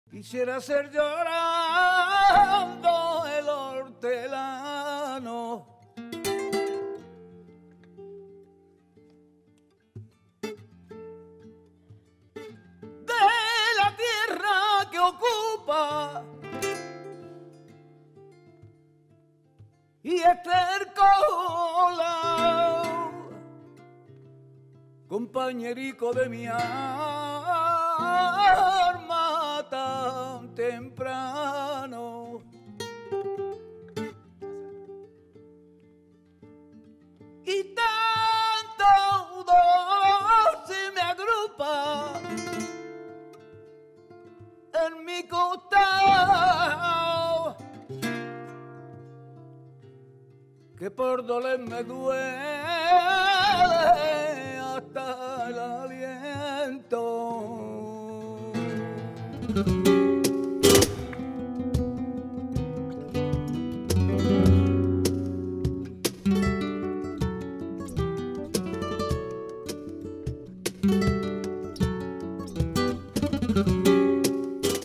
El Pele "TORERO FLAMENCO" (1/3)
La mítica figura del flamenco, el Pele, llega al Auditorio Nacional de Música con su espectáculo "El torero flamenco" donde mostrará todo el poderío de su voz. Su estilo es contundente y poderoso, perfectamente identificable, ya que no existe similitud con otros cantaores de su generación.